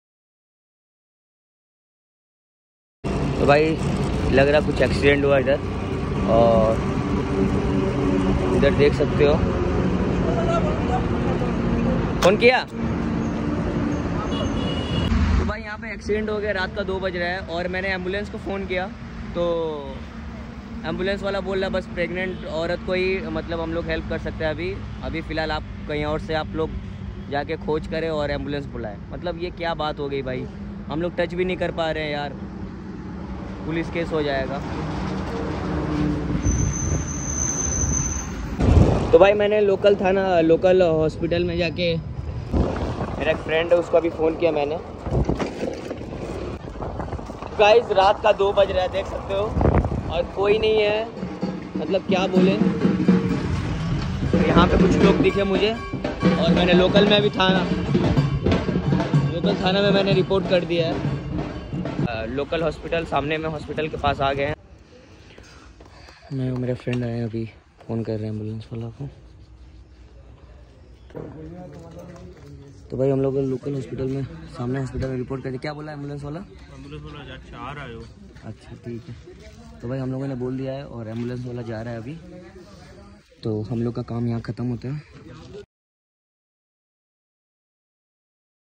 Raat ka 2bje accident || koi bhi help nhi kr raha tha 🥺
Plz aisi situation me aap log bhi help kre 🙏🏻